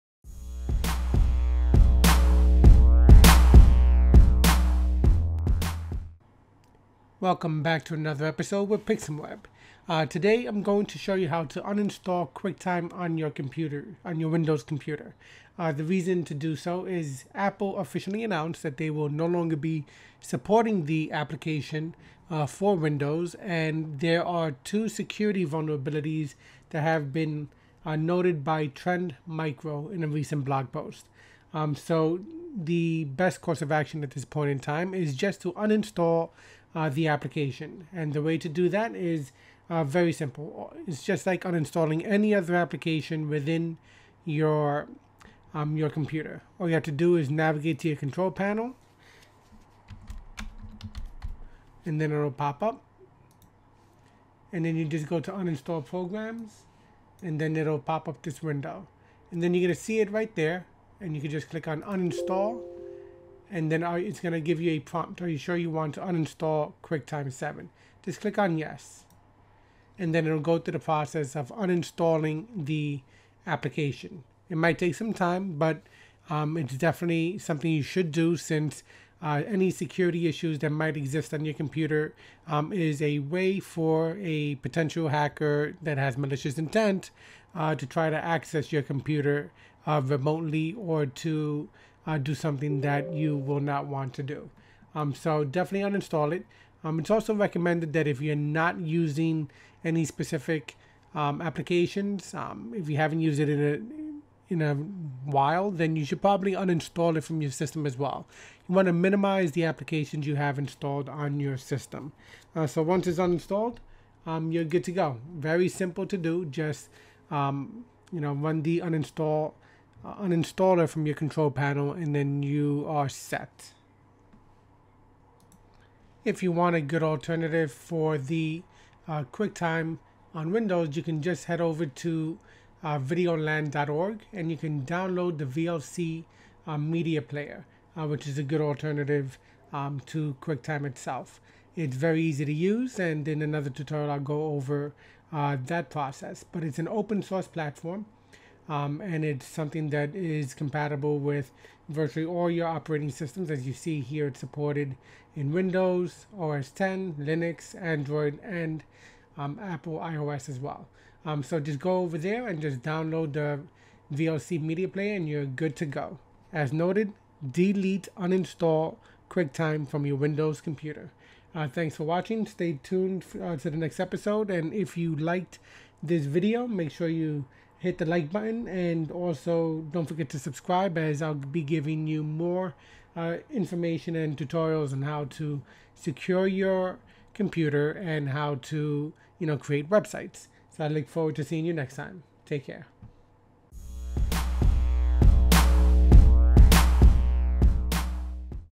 Uninstall QuickTime from Windows Computer - Security Alert
It's official, Apple is no longer supporting QuickTime for Windows. No more security updates or patches. This is concerning, especially since two security flaws have been identified which affect QuickTime for Windows. Best course of action is to uninstall QuickTime from your Windows Computer.